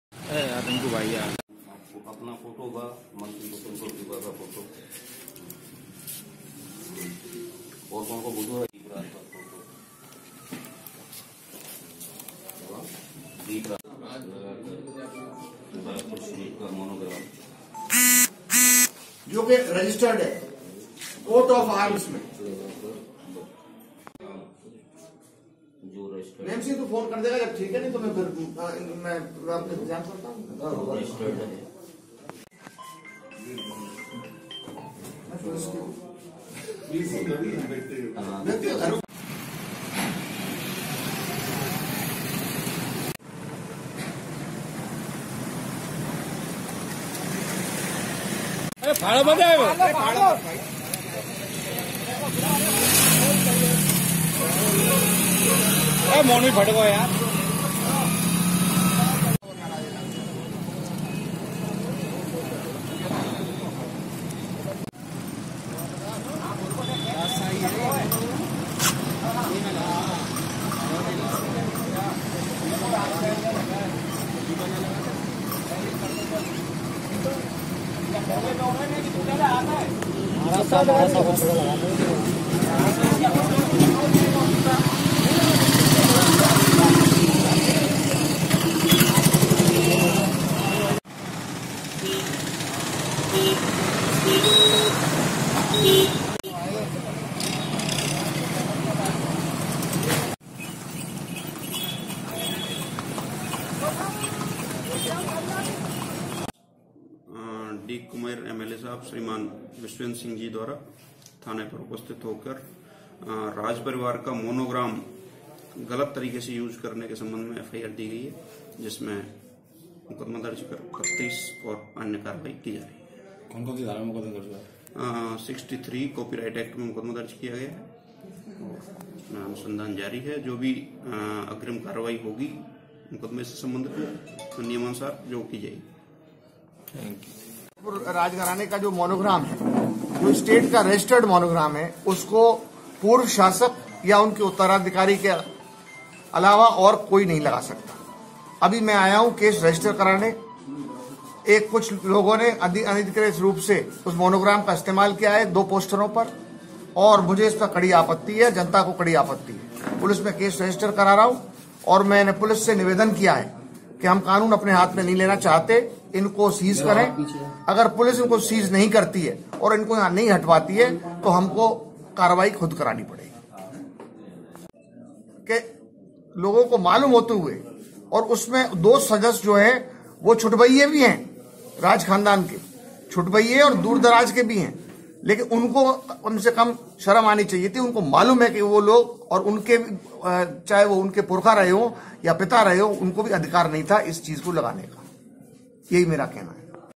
विश्वेन्द्र सिंह ने भाजपा पर रियासत का मोनोग्राम लगाने का मामला दर्ज कराया
भरतपुर के पूर्व राजपरिवार के उत्तराधिकारी विश्वेन्द्र सिंह ने बीजेपी नेताओं के खिलाफ रियासत का मोनोग्राम बैनर पर लगाने को लेकर कॉपी राइट के तहत मामला दर्ज कराया है. जिस पर पुलिस ने कार्रवाई करते हुए शहर में लगे बैनरों को हटवा दिया है. मथुरा गेट थाना प्रभारी राजेश पाठक ने बताया कि विधायक विश्वेन्द्र सिंह ने मोनोग्राम का उपयोग बैनर पर लगाने को लेकर कुछ लोगों के खिलाफ मामला दर्ज कराया था. जिस पर कार्रवाई की जा रही है. जानकारी के मुताबिक बीजेपी के कुछ नेताओं ने सूरज जाट सेना के शहर मे कई जगह बैनर लगाए थे... जिन पर बीजेपी नेताओं सहित राजपरिवार से संबंध रखने वाले दीपराज सिंह के फोटो और रियासत का मोनोग्राम लगा हुआ है.